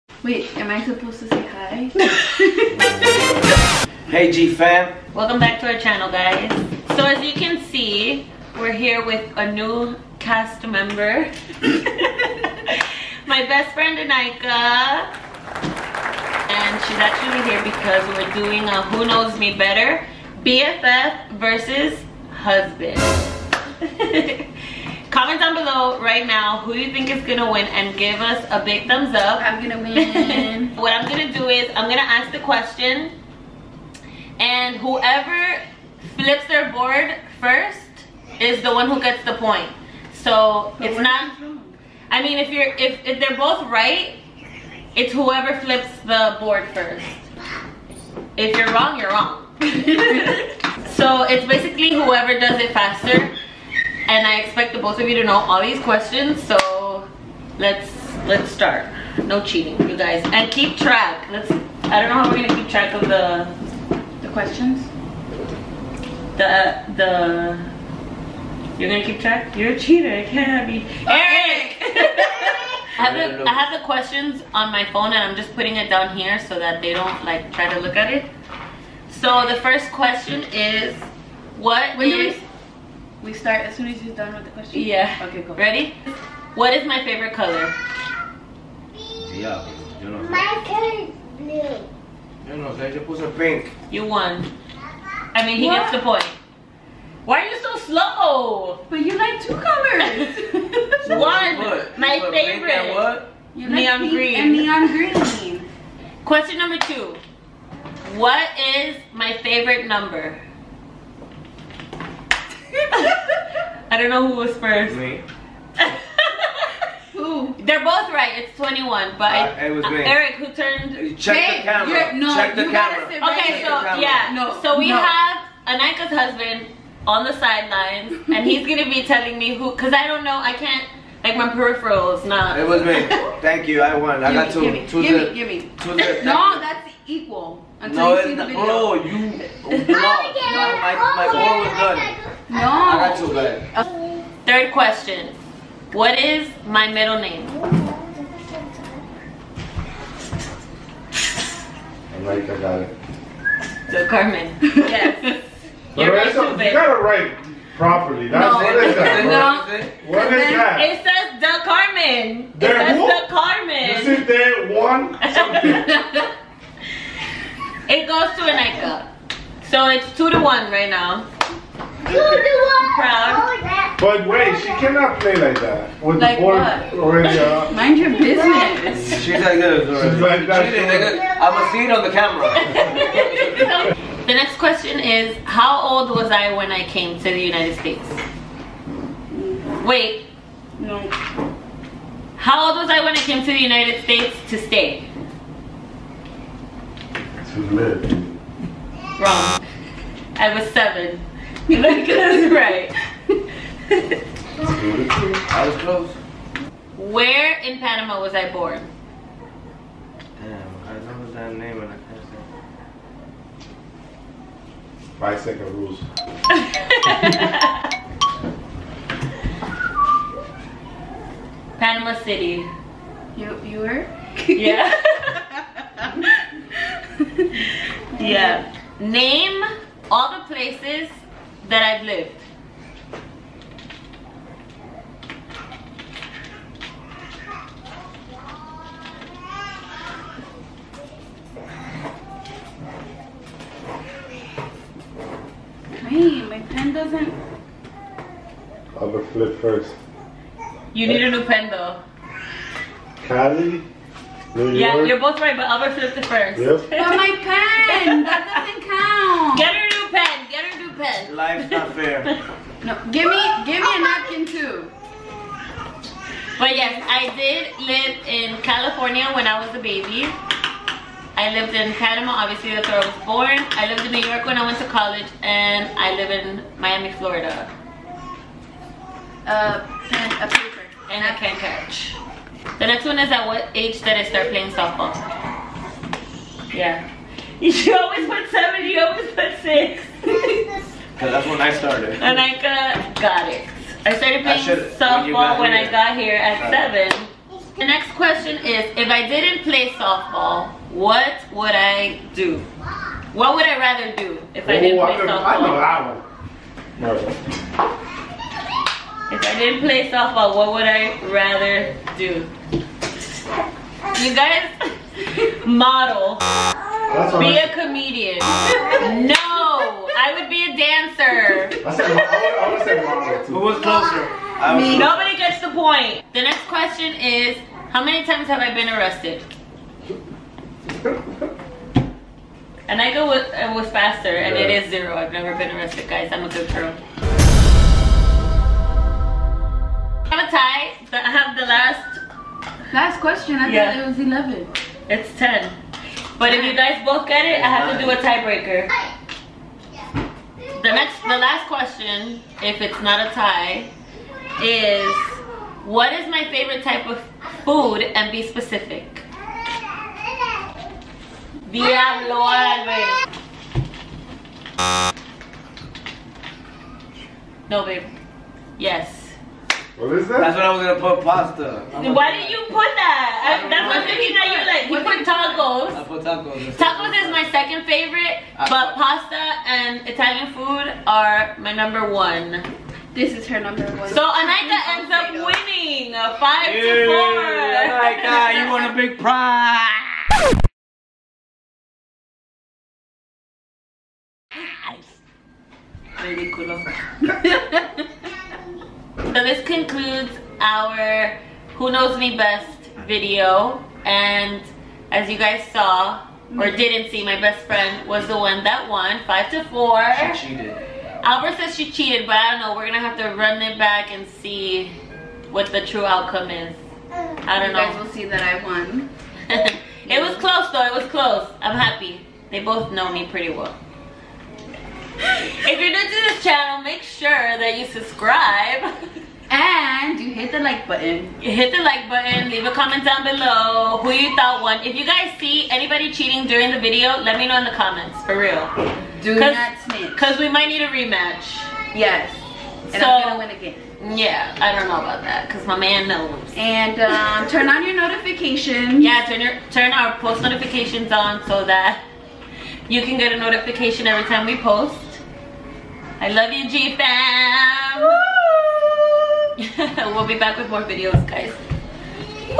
WHO KNOWS ME BETTER TAG HUSBAND OR BEST FRIEND!!! SHOCKING**
TODAY I FOUND OUT WHO KNOWS ME BETTER MY HUSBAND OR MY BESTFRIEND.

CHECK US OUT ON INSTAGRAM:
EVELYN @EVEDIABLO
ALBERT @AGONZ55
AVA @AVAGONZ
AJ @AJGONZ52